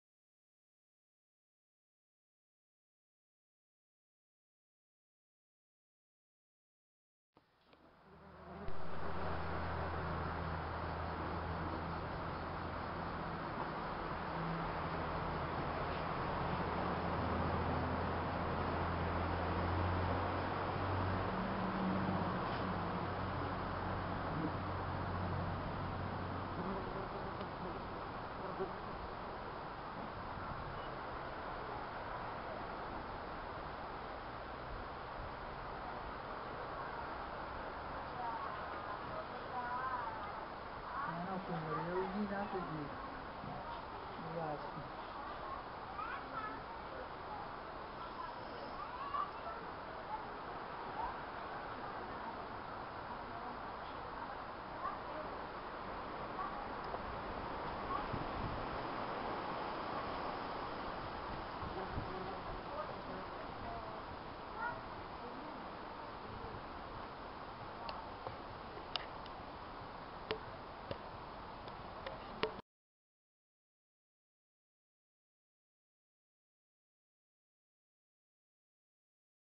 Was fressen Wespen: Wespen in Kroatien fressen Wienerwurst
Auch in Kroatien Essen Wespen von allem und jedem auf von  unserem Frühstückstisch.